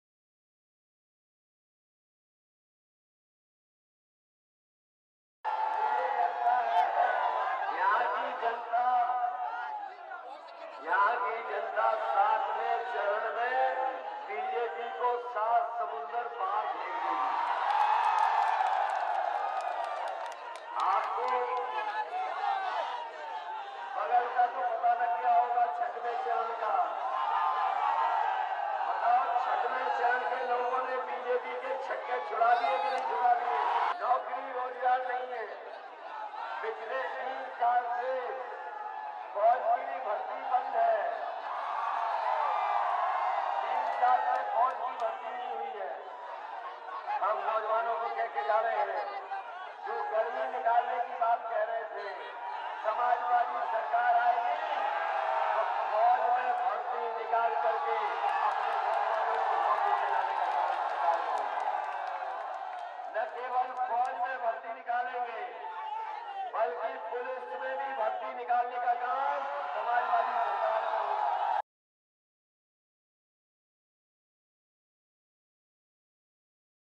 People of Azamgarh will send BJP across seven seas in seventh phase, says Akhilesh Yadav
Azamgarh (Uttar Pradesh), Mar 05 (ANI): Samajwadi Party Chief Akhilesh Yadav on March 05 said that the people of Azamgarh will send the BJP across seven seas in the seventh phase of the UP Assembly Elections. He also promised recruitments for Army and Police if elected to power. “Azamgarh people will send BJP 'Saat Samundar Paar' in the seventh phase of the UP Assembly Polls. When our government will come, along with police recruitments, we'll also ensure that recruitments for Army are done,” the chief said. UP is a poll-bound state. The results of the UP Assembly Elections will be declared on March 10. (KP)